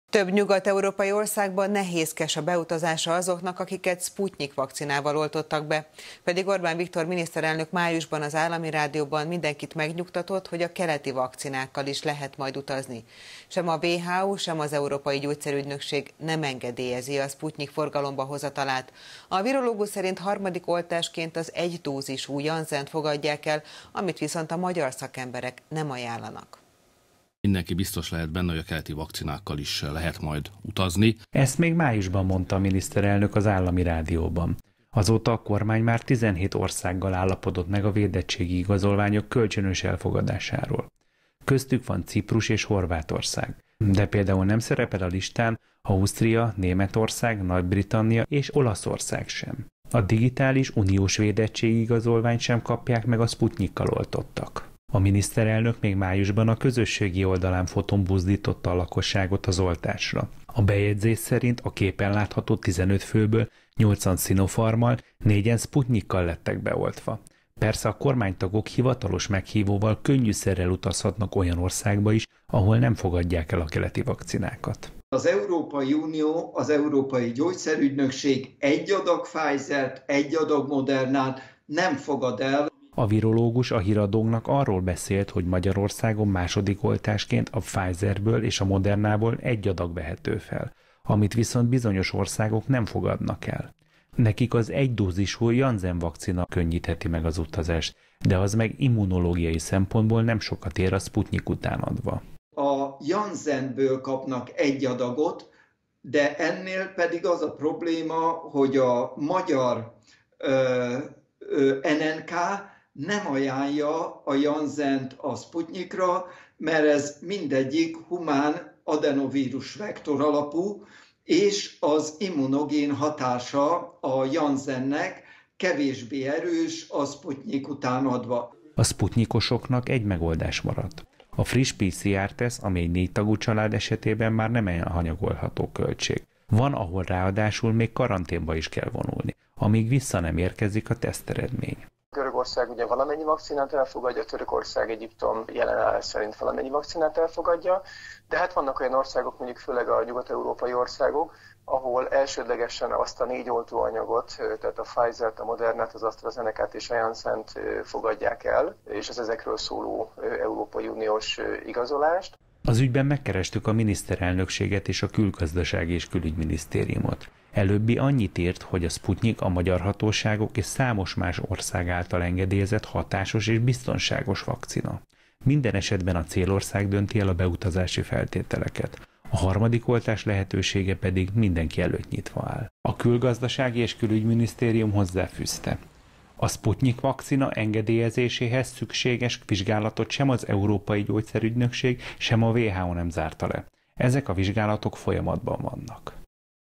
Virológus: A Szputnyikkal oltottak harmadik oltásnak Janssent kérjenek!
Több nyugat európai országba nehézkes a beutazása azoknak akiket, Szputnyik vakcinával oltottak be. Pedig Orbán Viktor miniszterelnök májusban az állami rádióban mindenkit megnyugtatott, hogy a keleti vakcinákkal is lehet majd utazni.